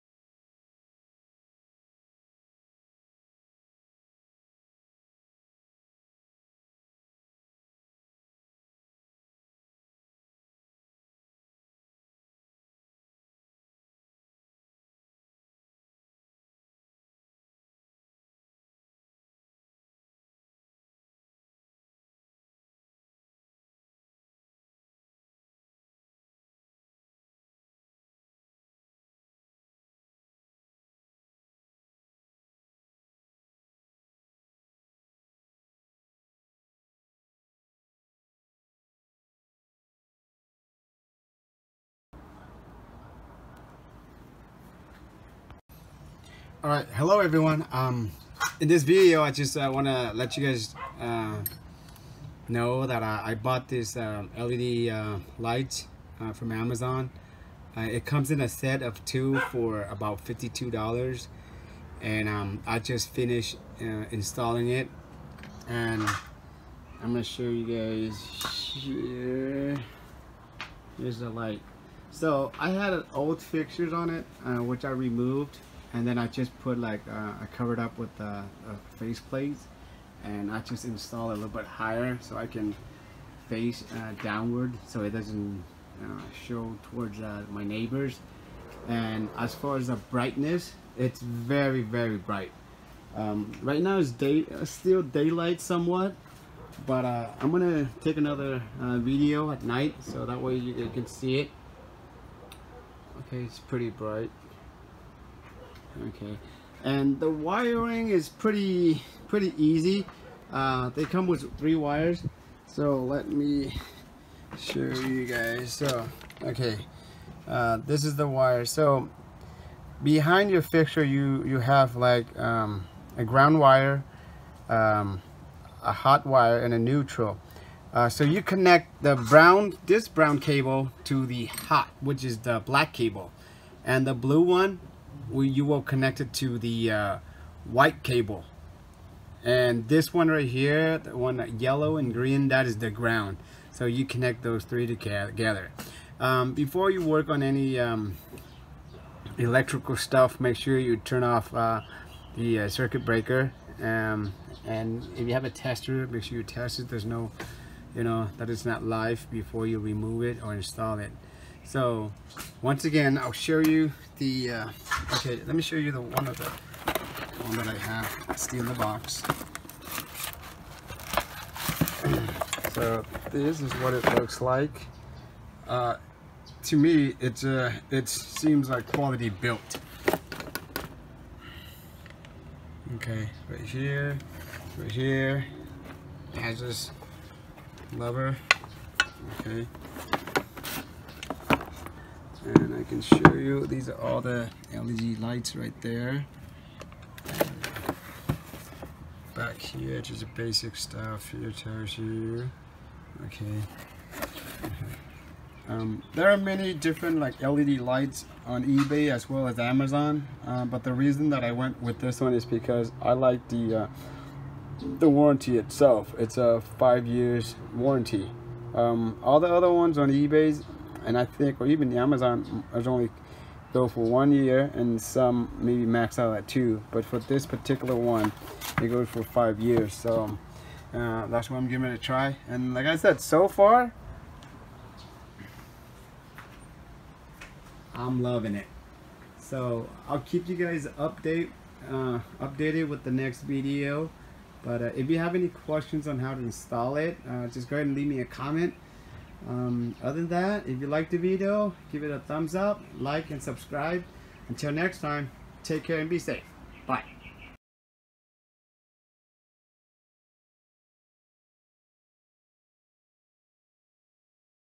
How to install Hyperikon LED flood light with the old fixture super bright
I have listed the link to Amazon to make it easy for anyone who wants to buy one.